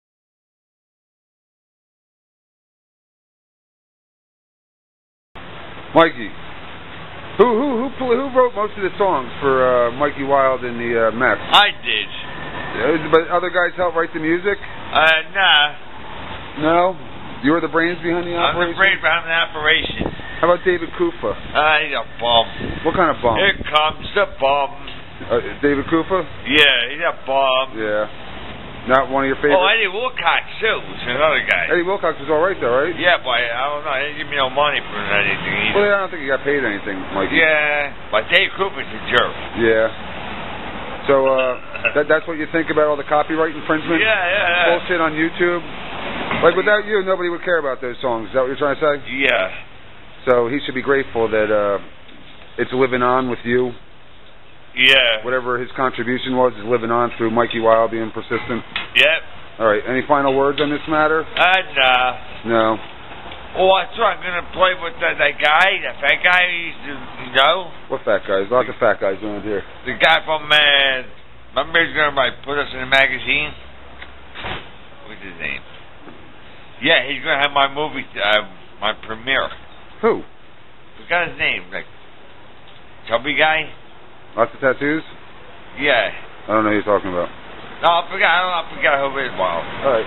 Mikey talks youtube
Mikey Wilds reaction about videos of him performing songs live getting flagged with youtube users being threatened with being kicked off of You tube for copy write violations.Basically,Mikey Wild and the 3rd Graders,the Mess,the Magic Lanterns whatever band backed him,wrote the Lyrics,the music did whatever,would have never amounted to anything with out Mikey fronting it or them,